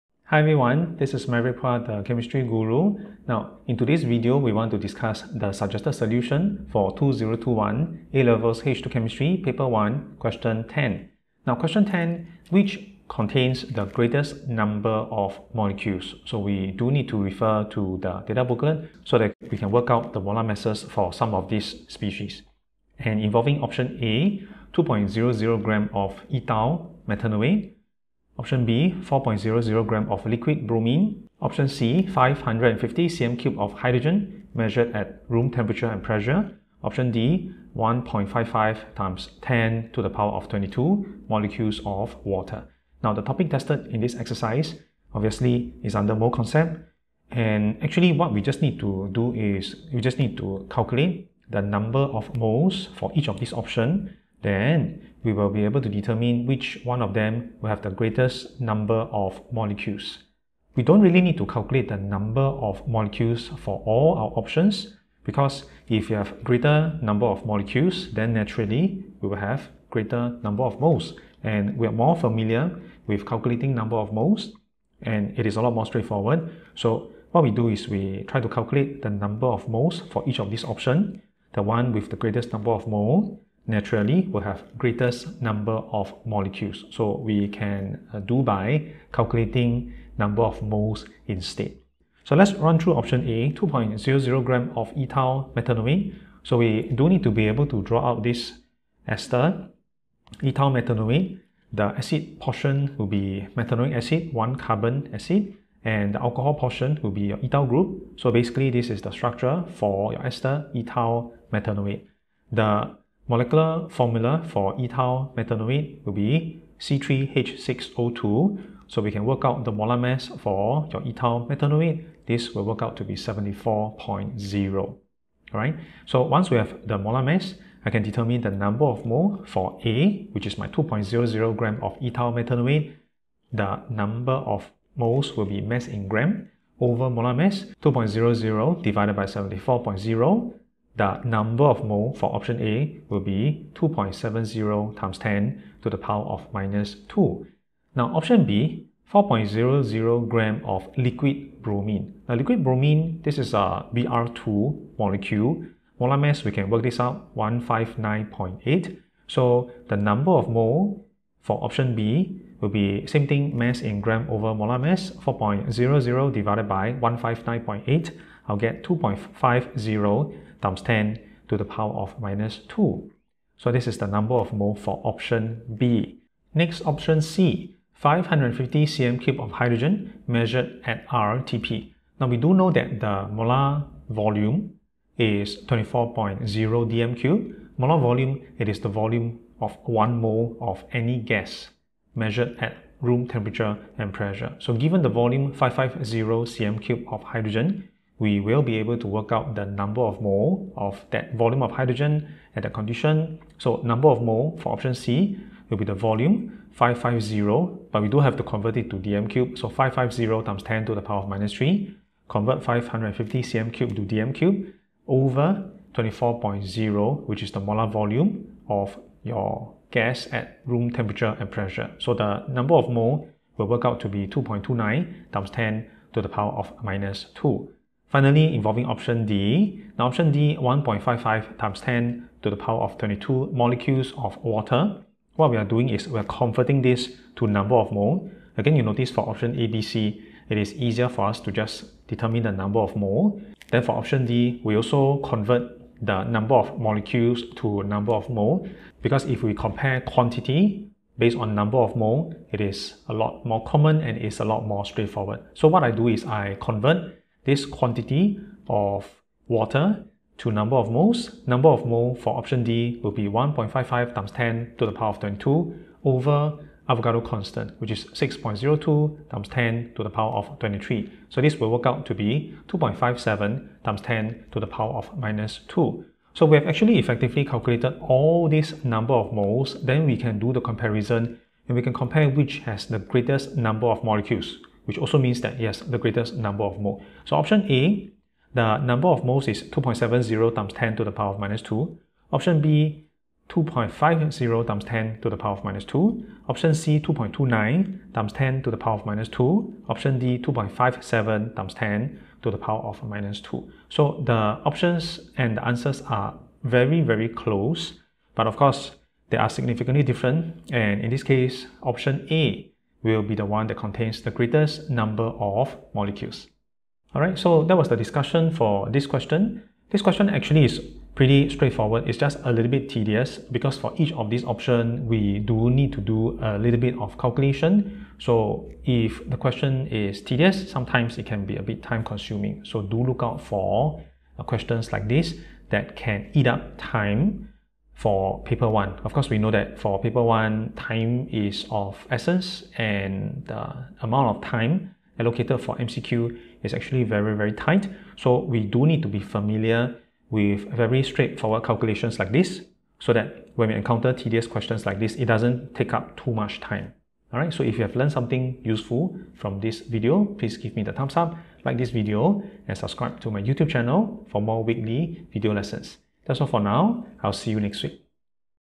2021 P1 Q10 - Determining Greatest Number of Molecules
Let's take a look at 2021 A Levels H2 Chemistry Paper 1 Question 10.

We are required to determine which option contains the greatest number of molecules.

Since number of molecules is directly related to number of moles, we can just calculate the moles for each option instead.

Option A - 2.00g of ethyl methanoate

The molar mass for ethyl methanoate, C3H6O2, is 74.0.

Number of moles will be mass over molar mass = 2.70 x 10e-2

Option B - 4.00g of liquid bromine

Molar mass for bromine, Br2, is 159.8.

Number of moles will be mass over molar mass = 2.50 x 10e-2

Option C - 550cm3 of hydrogen measured at rtp

Molar volume of a gas at room temperature and pressure is 24.0dm3.

Number of moles will be volume in dm3 over molar volume = 2.29 x 10e-2

Option D - 1.55 x 10e22 molecules of water

Number of moles will be number of molecules over Avogadro constant L = 2.57 x 10e-2

We can now compare all the options to determine which has the greatest moles (and greatest number of molecules).

The answer to this question is option A (2.70 x 10e-2).

This question is not difficult but can be pretty tedious.

Hence we have to be familiar with our mole concept calculations to attempt this question efficiently.

Topic: Mole Concept, Physical Chemistry, A Level Chemistry, Singapore